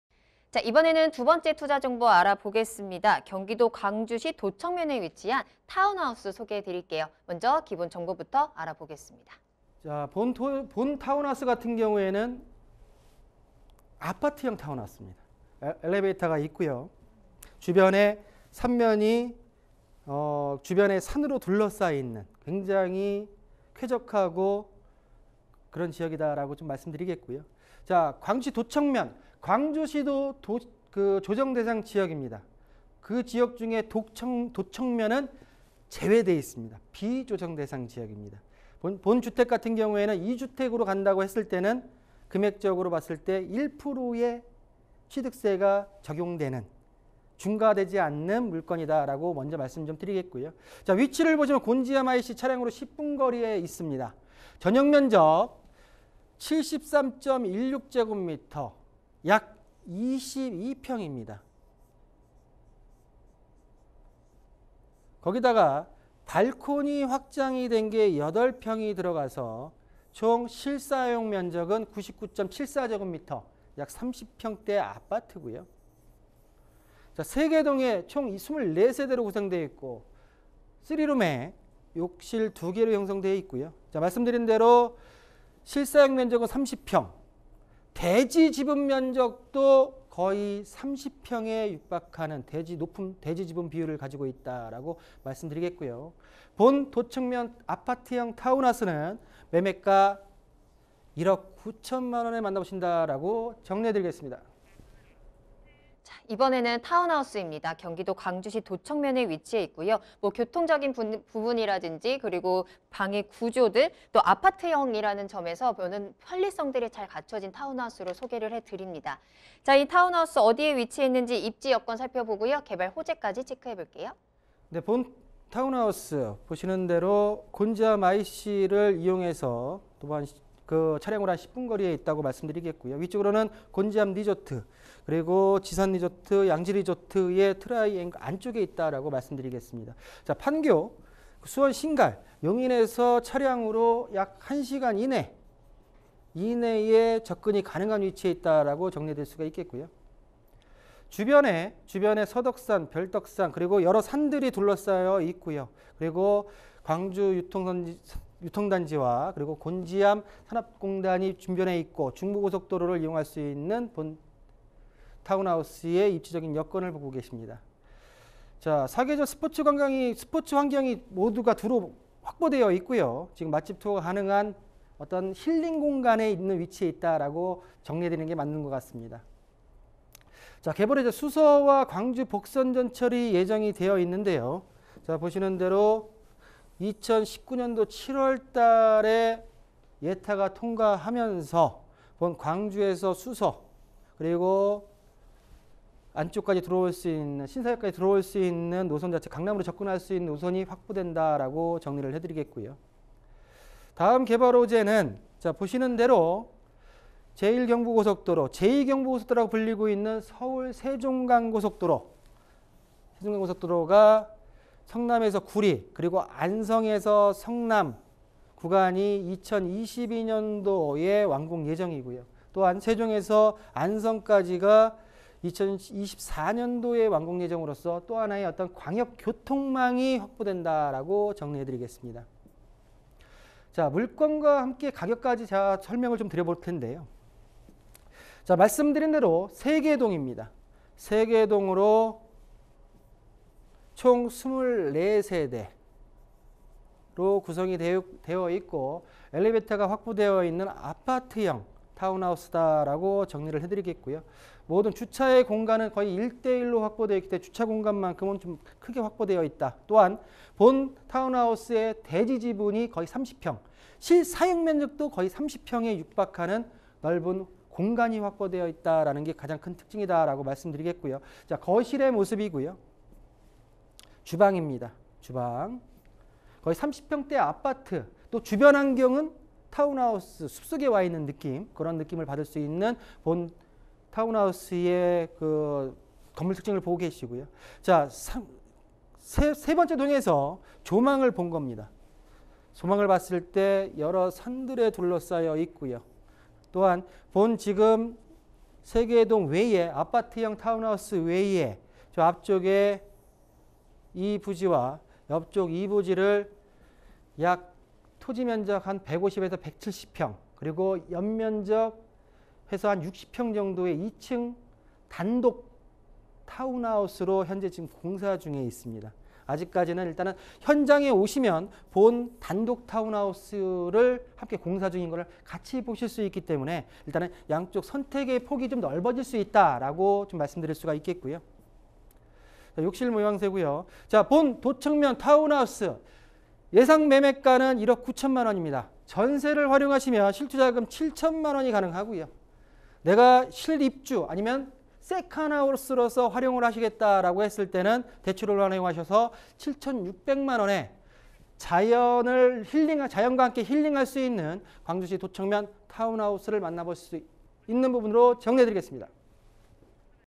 부동산시그널 : 자연친화적 힐링 세컨하우스 ‘광주시 도척면 타운하우스’
박성규 대표(JYP홀딩스)의 추천 투자정보
광주시 도척면 타운하우스

[투자포인트]
▶ 수서~광주 복선전철 예정(2019년 7월 예타 통과)
▶ 경기권 3대 스키장 인접 - 숙소로 이용 가능
▶ 서울~세종 고속도로(성남~구리간) 2022년 개통 예정

생방송 -부동산시그널- 수, 금 오전 11:00~12:00